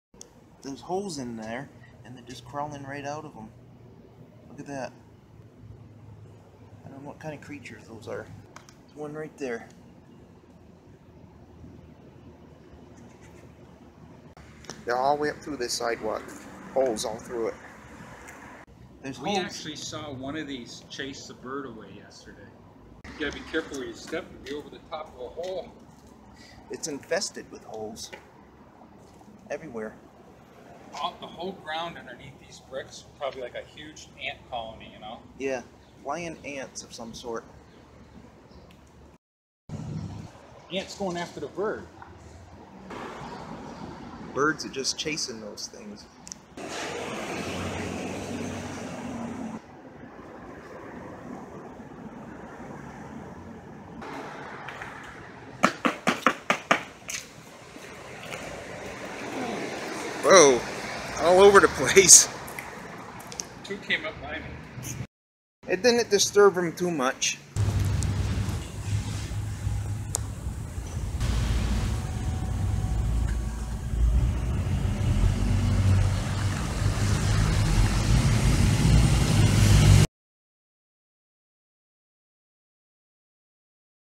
Strange Flying Insects Infestation Under Sidewalk
Whole entire walkway infested underneath with ant like flying insects. They were chasing the birds and the birds were chasing them. Next week back to my regular silly videos.